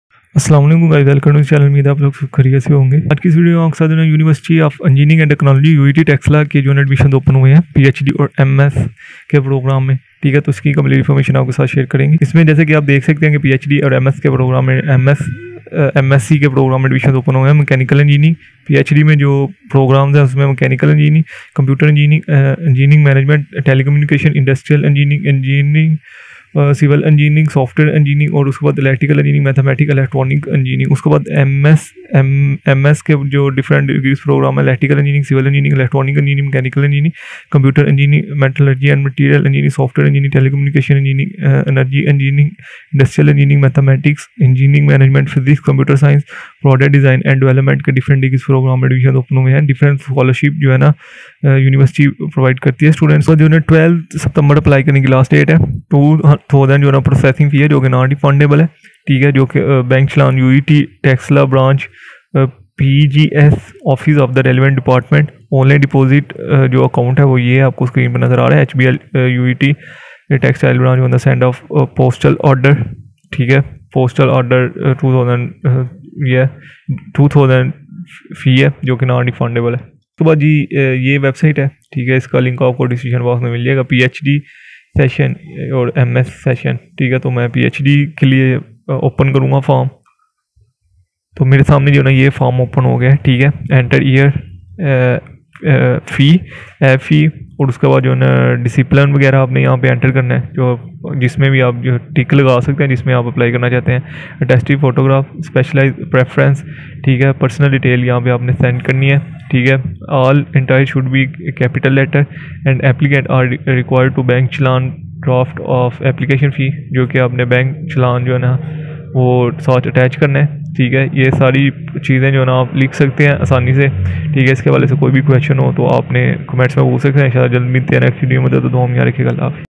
UET Taxila MPhil PhD Admissions Fall 2022 : How to Apply :Postgraduate MPhil and PhD admissions
Rs info Media